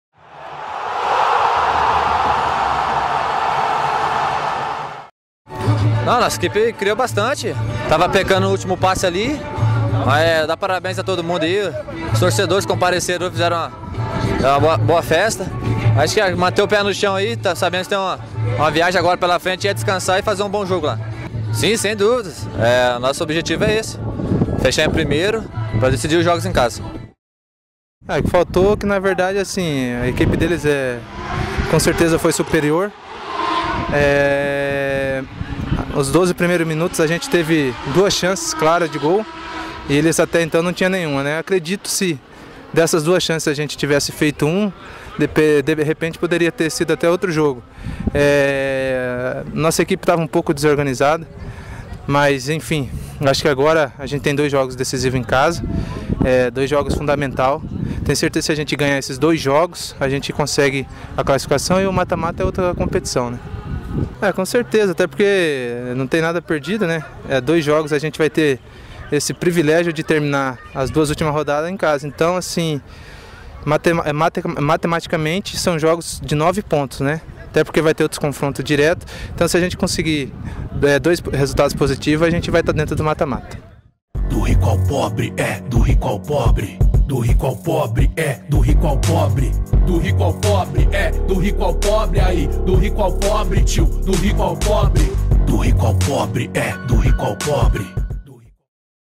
[CP] - SÃO JOSEENSE 3 X 0 VERÊ [7ª RODADA | 3ª DIVISÃO PARANAENSE 2017]
Confira as entrevistas realizadas na última quarta (1) no Estádio do Pinhão após a partida entre São Joseense e Verê Futebol Clube pela sétima rodada da Terceira Divisão Paranaense.

Com a palavra, o atacante do Independente Robinho e o volante Rubro Verde Casemiro.